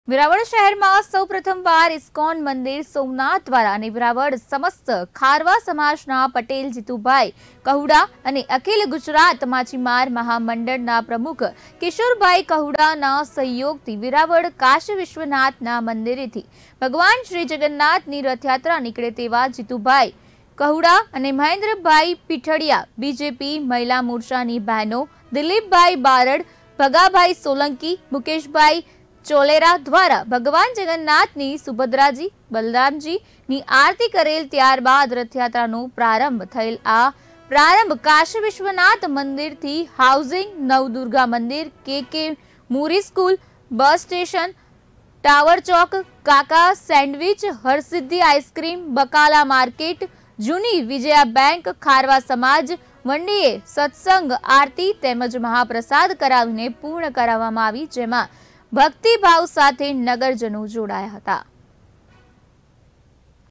વેરાવળમાં સૌ પ્રથમવાર નીકળી ભગવાન જગન્નાથ યાત્રા
ભગવાનની યાત્રામાં શહેરીજનો, અગ્રણીઓ રહ્યા ઉપસ્થિત
યાત્રામાં શહેરીજનો, અગ્રણીઓ અને સાધુ સંતો રહ્યા ઉપસ્થિત
મહાપ્રસાદ કરાવીને જગન્નાથ યાત્રા પૂર્ણ કરવામાં આવી